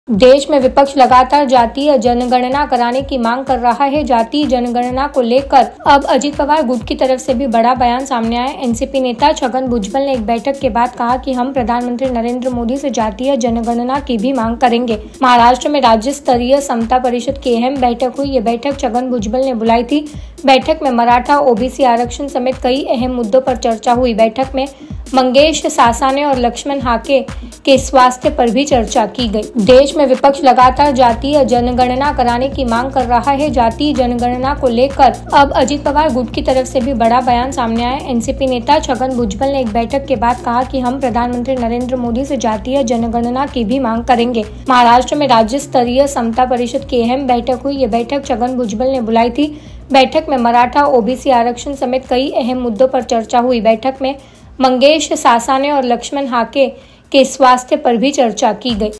Chagan Bhujbal ने जातीय जनगणना को लेकर बड़ा बयान दिया, पीएम मोदी से मांग करेंगे
देश में विपक्ष लगातार जातीय जनगणना कराने की मांग कर रहा है. जाति जनगणना को लेकर अब अजित पवार गुट की तरफ से भी बड़ा बयान सामने आया है. एनसीपी नेता छगन भुजबल ने एक बैठक के बाद कहा कि. "हम प्रधानमंत्री नरेंद्र मोदी से जातीय जनगणना की भी मांग करेंगे."महाराष्ट्र में राज्य स्तरीय समता परिषद की अहम बैठक हुई. ये बैठक छगन भुजबल ने बुलाई थी. बैठक में मराठा, ओबीसी आरक्षण समेत कई अहम मुद्दों पर चर्चा हुई. बैठक में मंगेश सासाने और लक्ष्मण हाके के स्वास्थ्य पर भी चर्चा की गई.